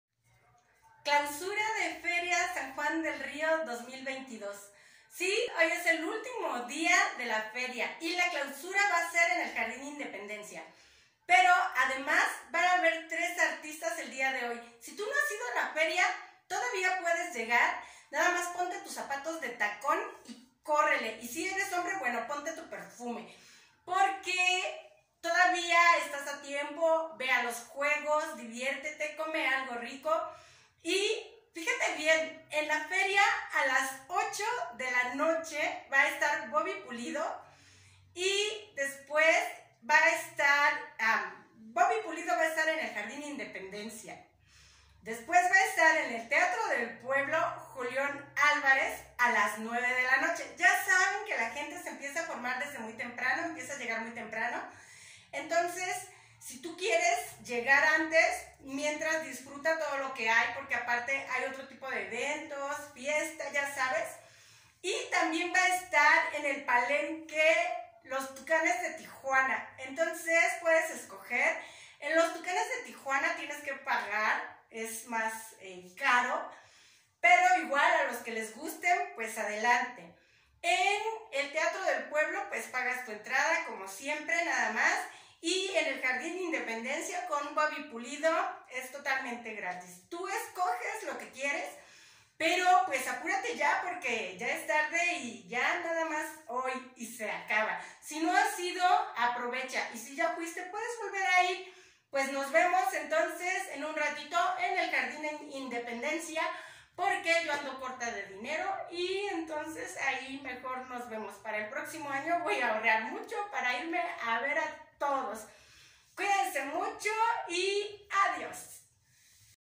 feria San Juan del Río habrá 3 artistas, día de clausura
Feria San Juan del rio 23 día de clausura Tres artistas, ve quien y donde estarán, no te lo puedes perder